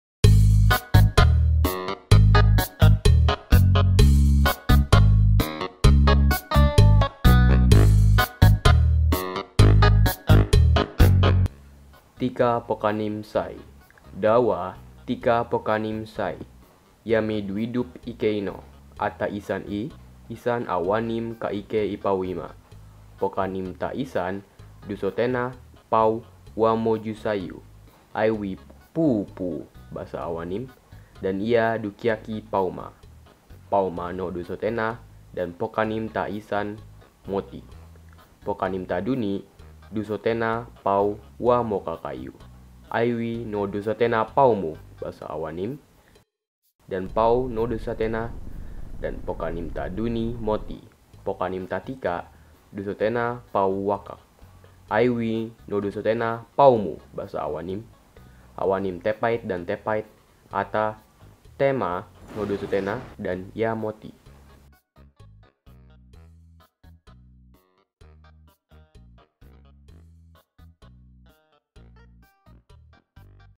3 Little Pigs in Englonesian (ENI/Basa Pien) Auxilliary Conlang | Language
i'll do a video explaining the language later.
also if it sounds similar to indonesian it's because indonesian is one of the main languages that is used to make this language.